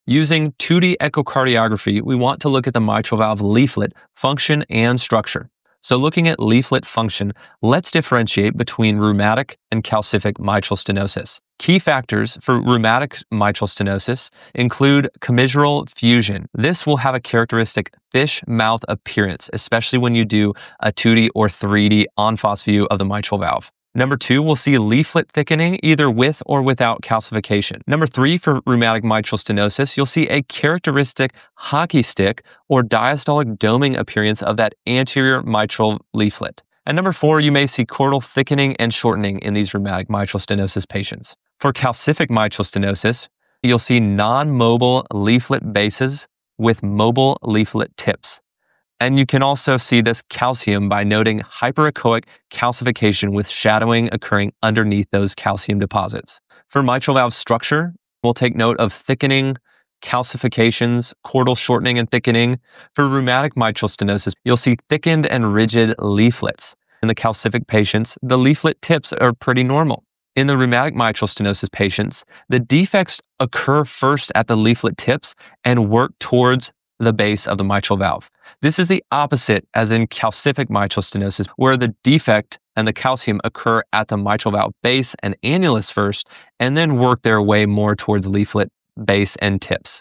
Differences between Rheumatic and Calcific Mitral Stenosis on TEE
(CLIP FROM TEE TIME EPISODE 11

Train your eye to distinguish rheumatic from calcific mitral stenosis with 2D echocardiography. This clip reviews classic findings: fish-mouth valve, hockey-stick leaflet motion, and base-to-tip vs. tip-to-base calcification patterns.